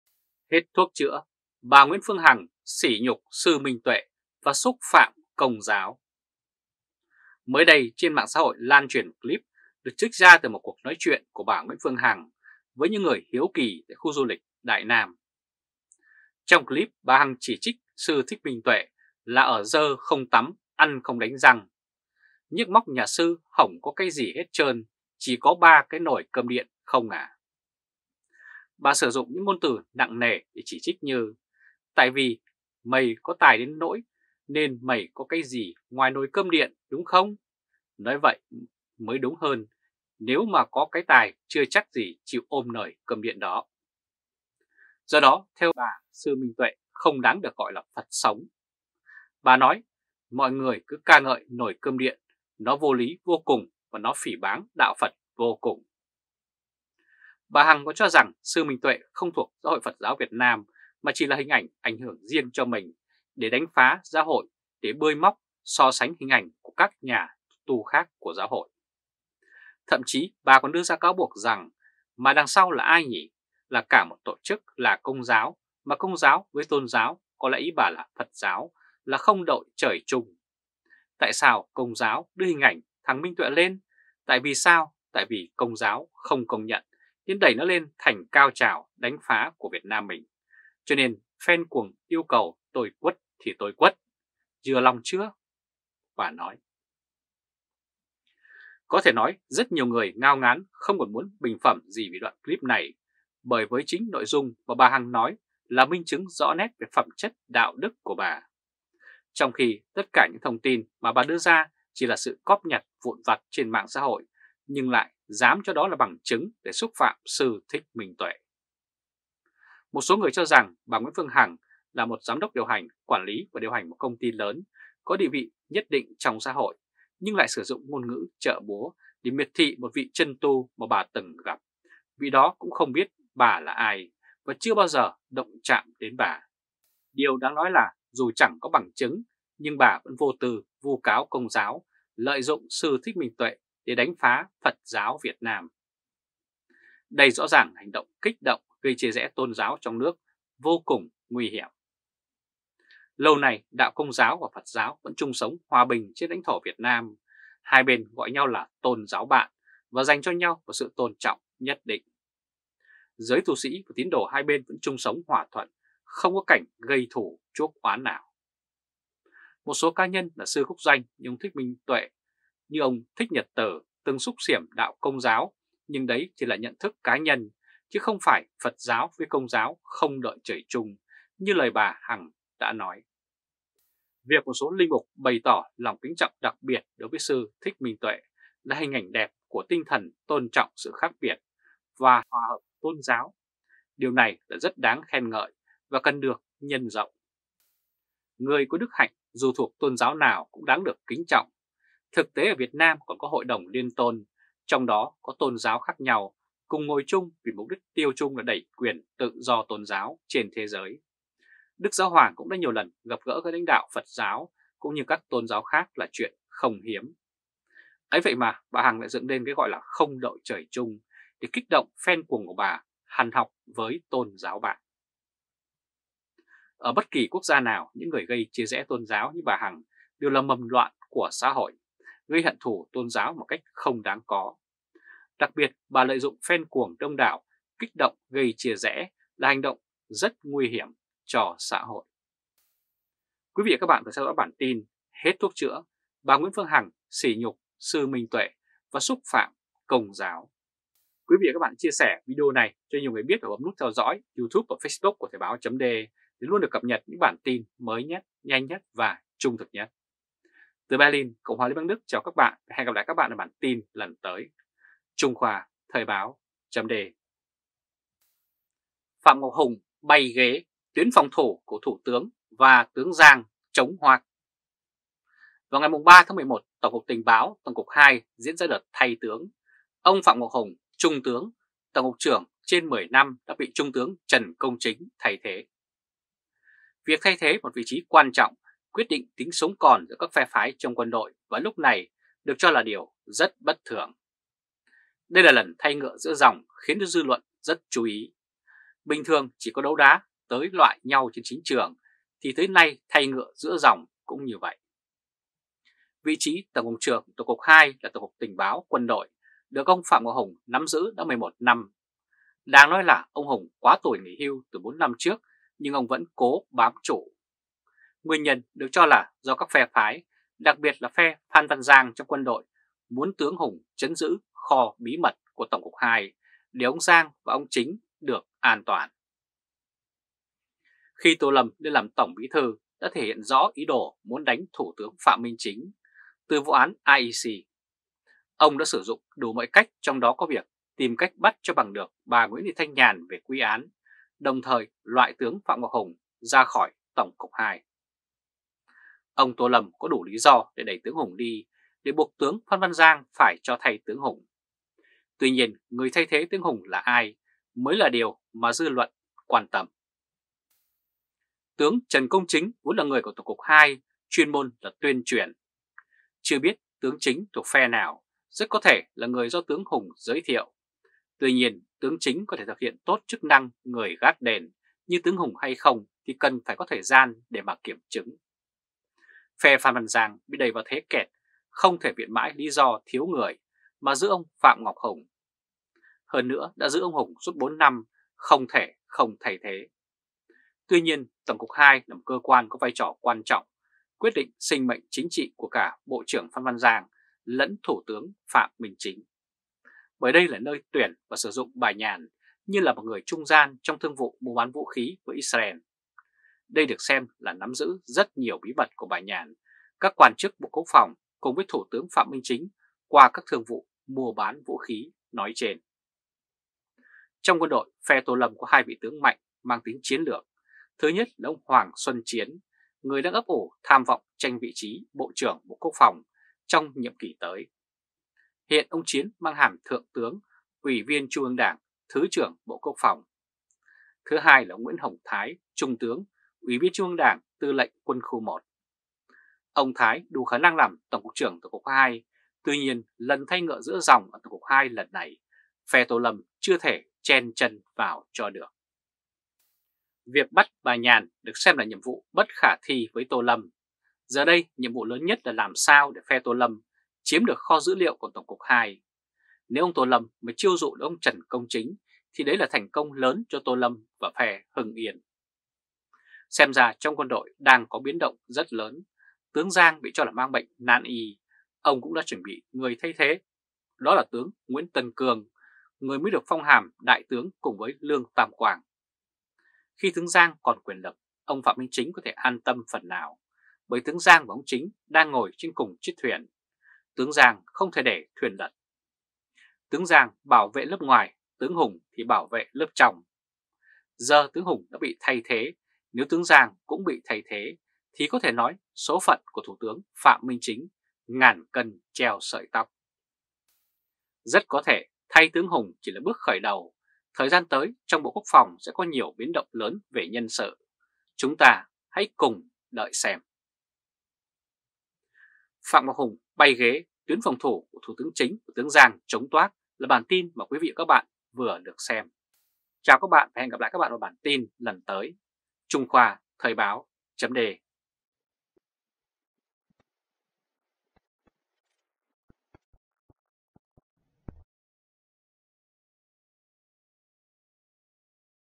Hết thuốc chữa! Bà Nguyễn Phương Hằng sỉ nhục sư Minh Tuệ và xúc phạm Công Giáo!
Ở bất kỳ quốc gia nào, người gây chia rẽ tôn giáo như bà Nguyễn Phương Hằng là mầm loạn của xã hội. Gây hận thù tôn giáo một cách không đáng có. Đặc biệt, bà Hằng đang lợi dụng fan cuồng đông đảo để kích động thì rất nguy hiểm cho xã hội.
Xem bài chi tiết tại:
Hoặc: